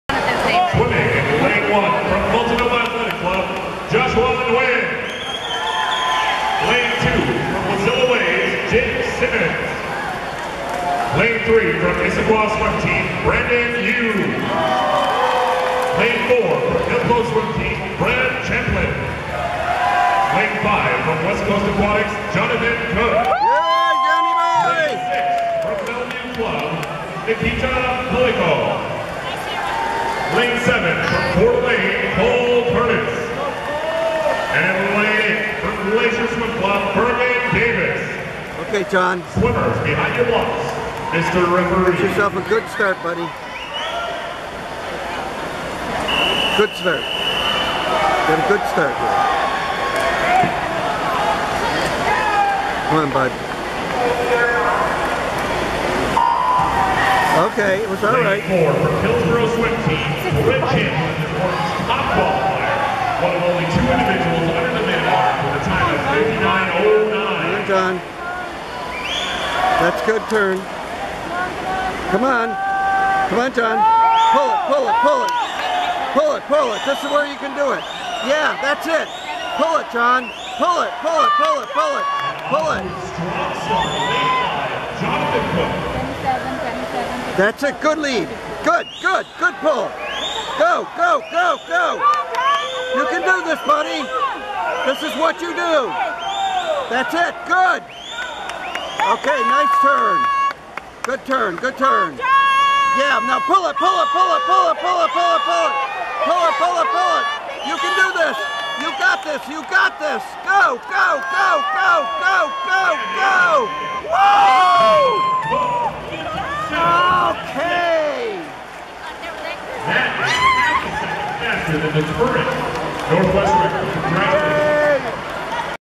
100 Breast Final
2015 Northwest Age Group Regions Short Course Championship
100 Breast Finals - 57.26